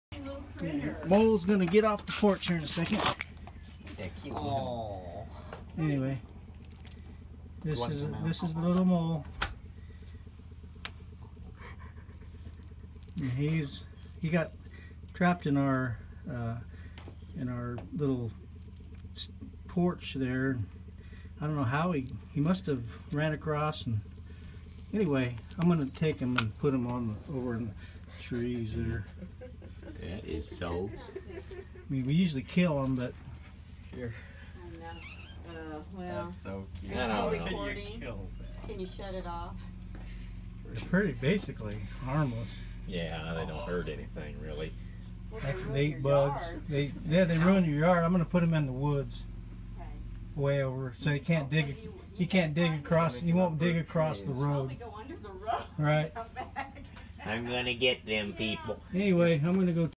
Mr Mole
A mole I found in our basement stairwell.   At one point you can hear it squeak.